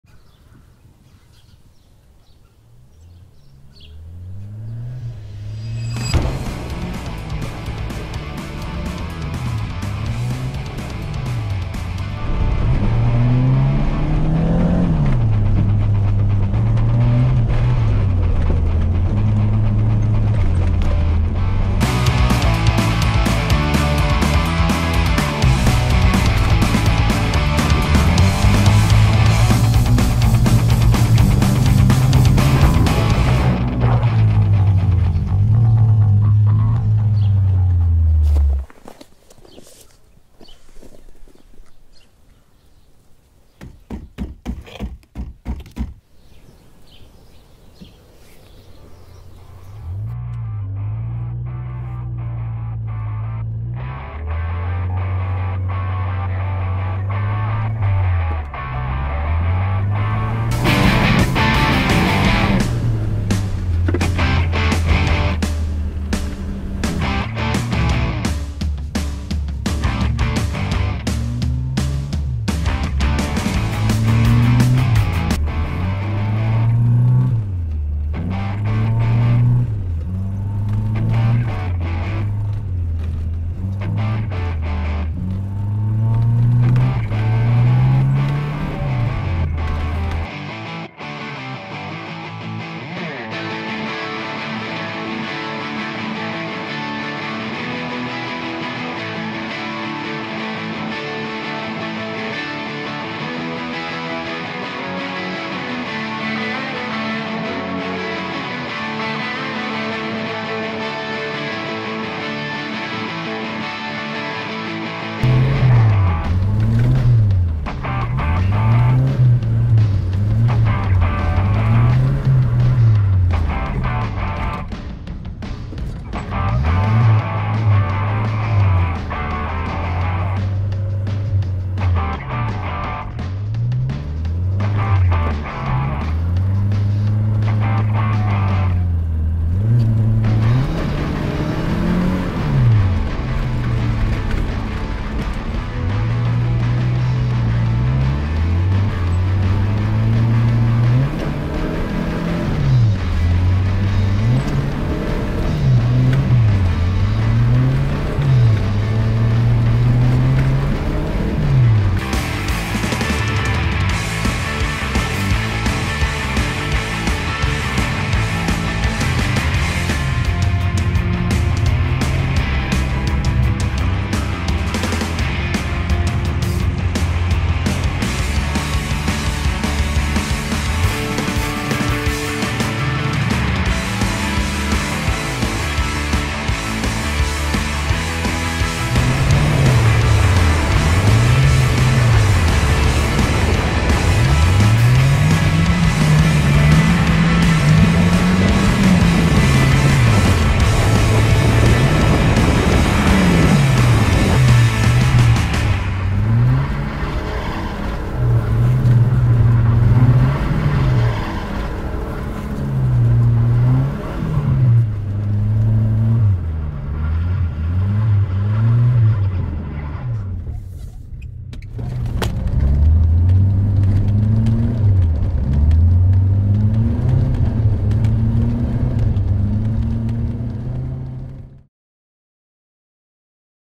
E30 WINTER WONDERLAND FUN
Enjoying winter fun in forest with my e30